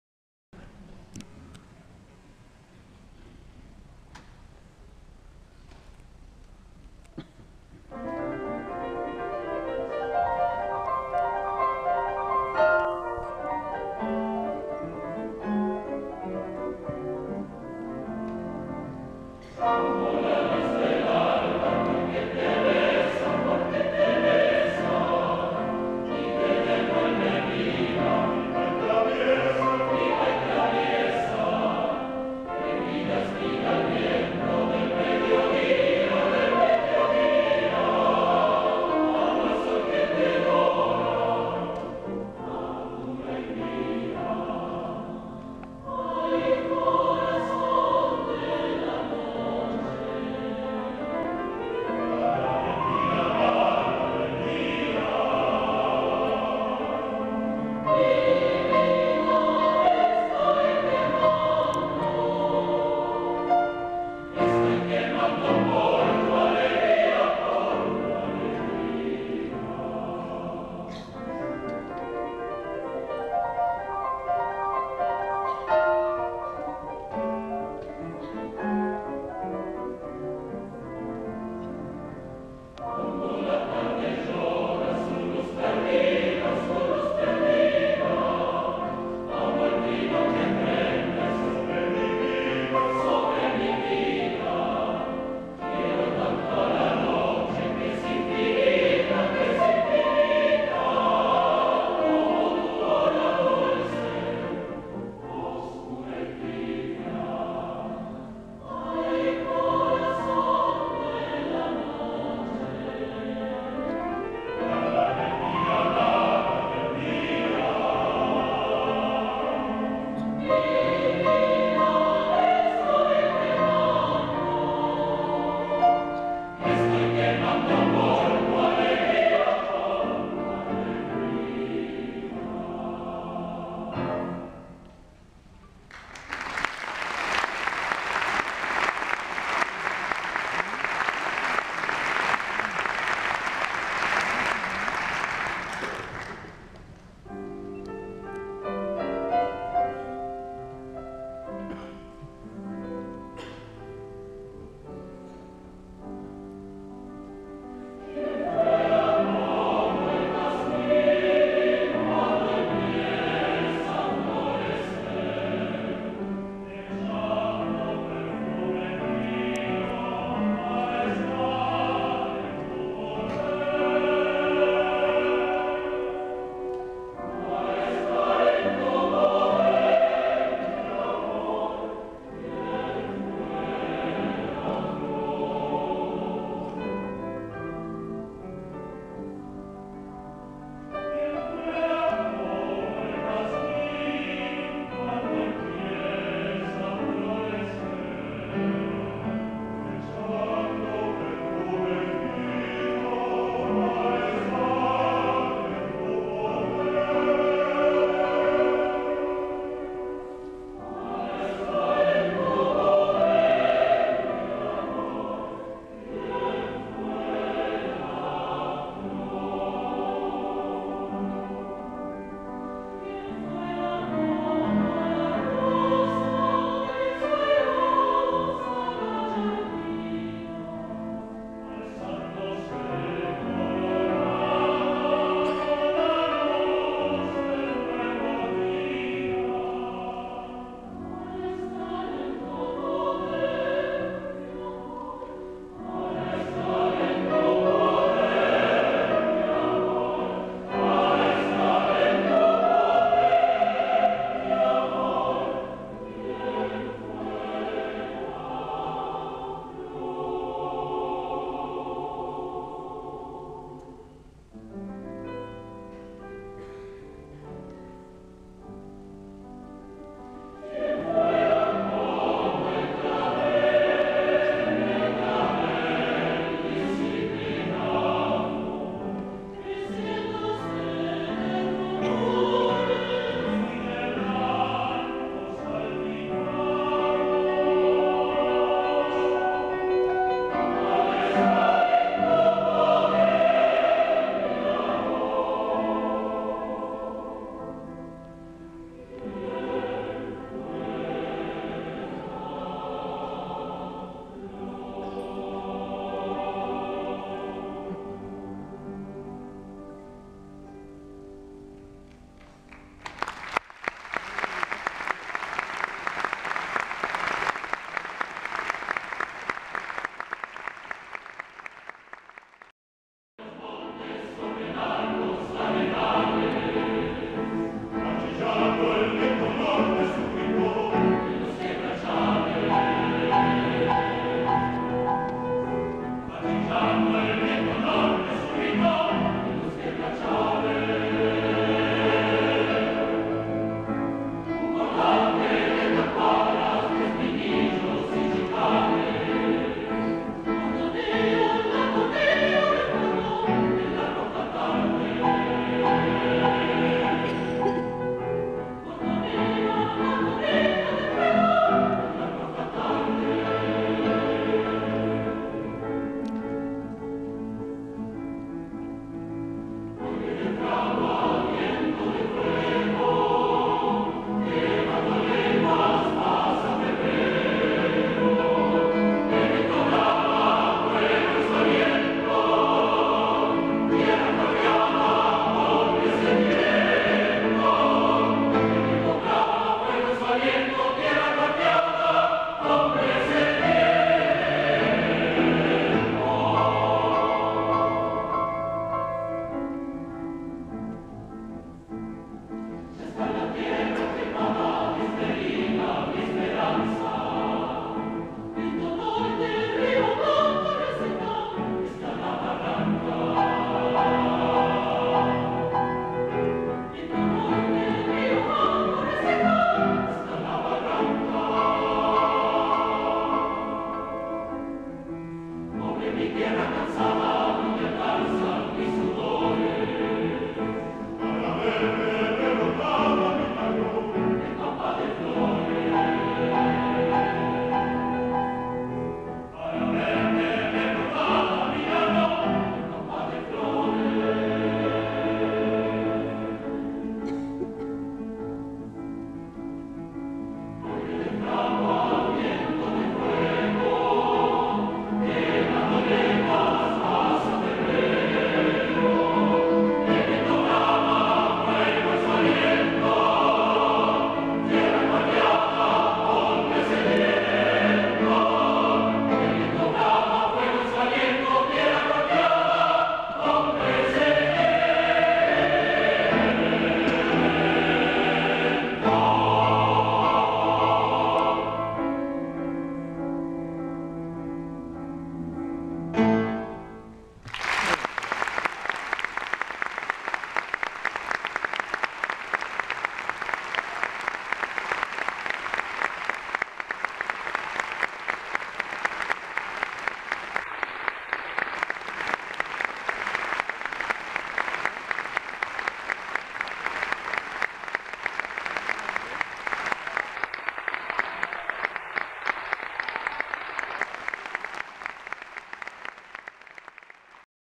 Coro Polifónico Nacional-Velada de Gala CCK-24-05-2017 @!
En la víspera del aniversario de la Revolución de Mayo, la Orquesta Sinfónica Nacional y el Coro Polifónico Nacional ofrecieron un concierto especial, con un repertorio íntegramente argentino, de obras de Ginastera, Guastavino, Gianneo y J. J. Castro.
Orquesta Sinfónica Nacional - Director Invitado: Federico Sardella /
Coro Polifónico Nacional - Director: Ariel Alonso
" VIVA LA PATRIA CARAJO " !!!...